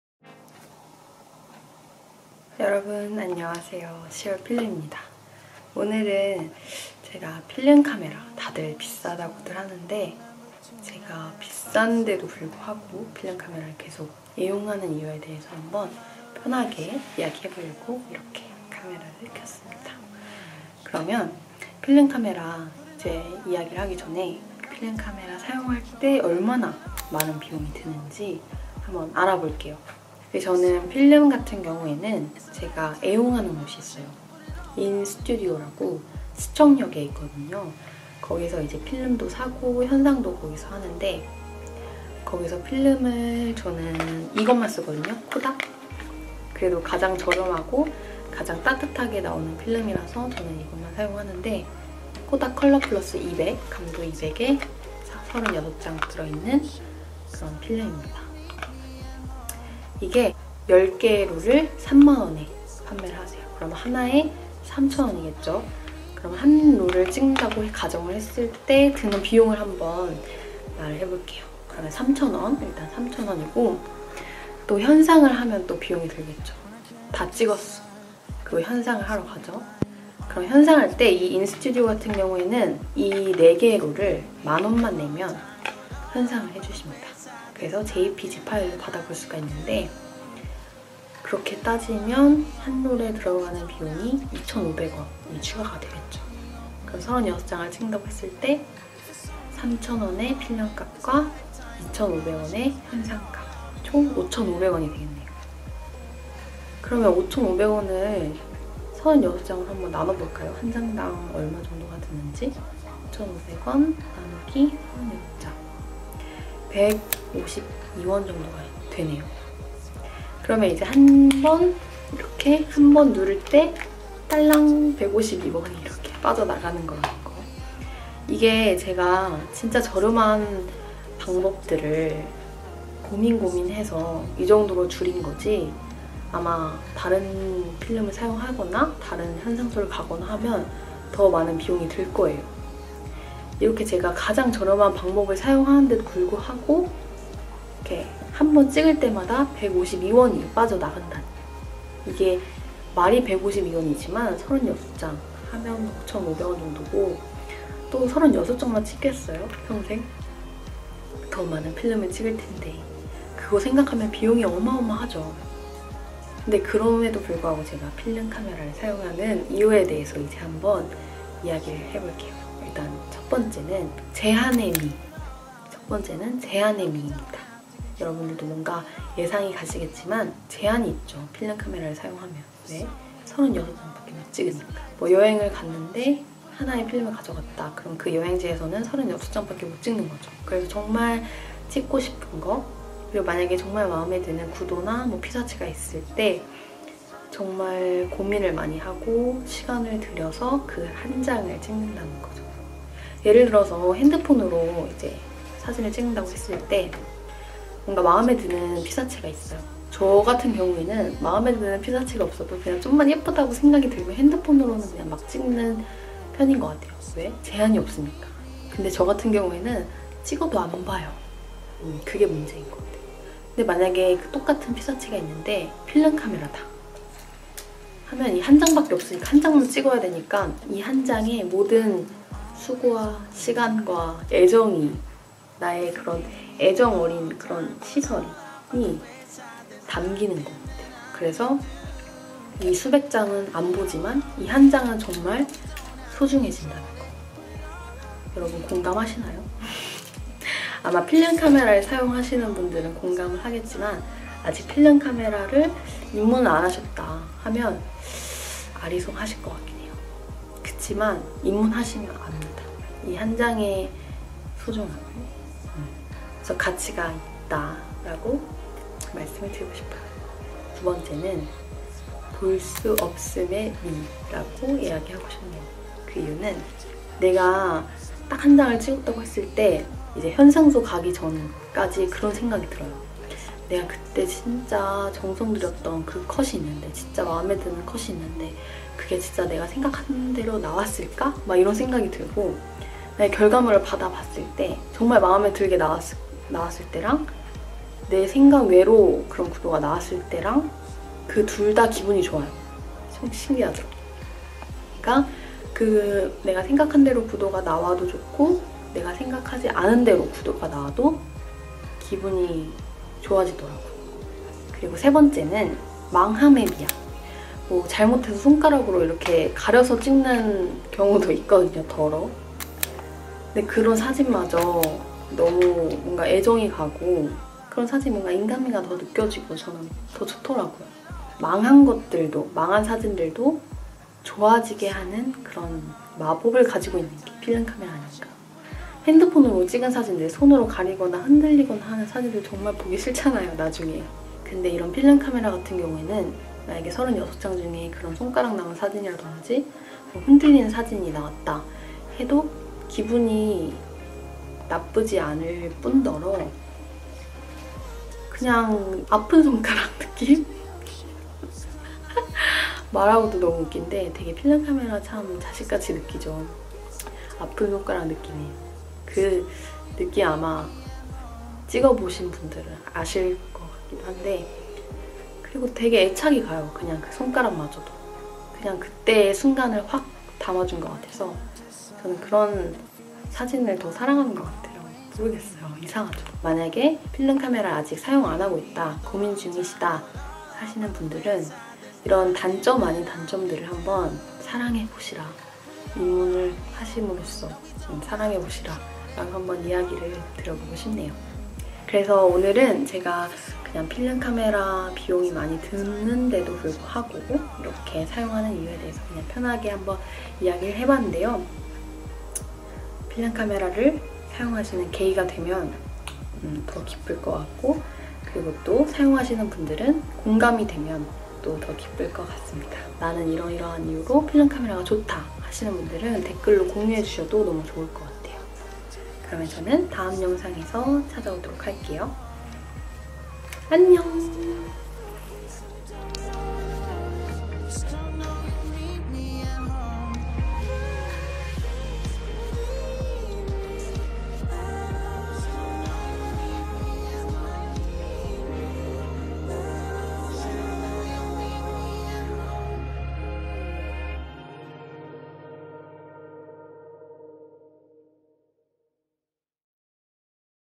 필름카메라, 비싸도 포기 못하는 이유???
여러분, 안녕하세요 :) 시월필름입니다!

필름 영상은 정말 오랜만이죠? ㅎㅎ
오늘은 필름카메라 비싸다고들 하는데..! 그럼에도 끊을 수 없는 이유에 대해서 이야기 해봤어요!

여러분들은 필름카메라를 고집하는 이유! 혹시 있나요? 있다면, 공유해주세요!

앞으로 여행/필름/우쿨렐레 영상으로 꾸준히 찾아올게요! :))) S2
(+댓글을 남겨주시는 감사한 구독자분들을 다음 영상 Thanks to에 올려드려요!)

오늘도 영상 봐주셔서 감사합니다 S2

+필름가격이 최근에 50000원으로 인상했다고 하네요 ㅠㅠ! 그리고 요즘 재고도 많이 부족한가봐요.. 혹시 인스튜디오 가시는 분들은 미리 전화로 확인 후 방문해주시면 좋을 거 같아요!